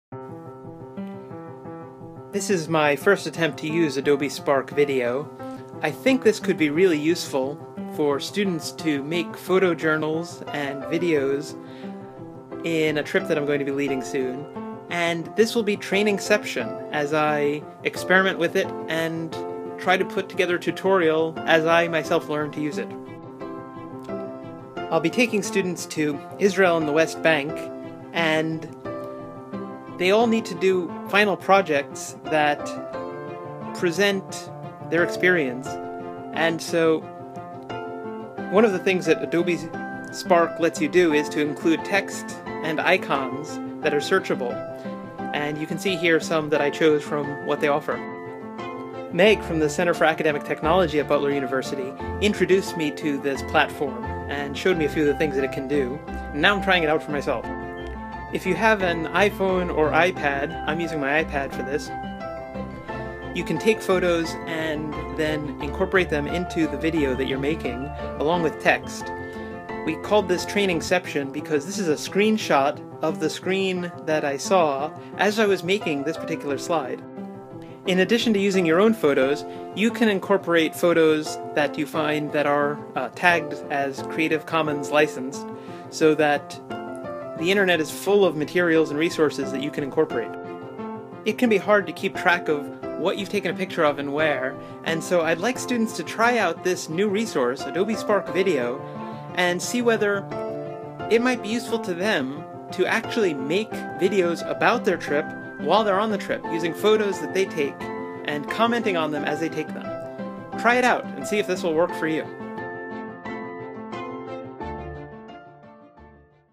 Introducing Adobe Spark Video
A tutorial and trial run of this very useful app, by James McGrath of Butler University.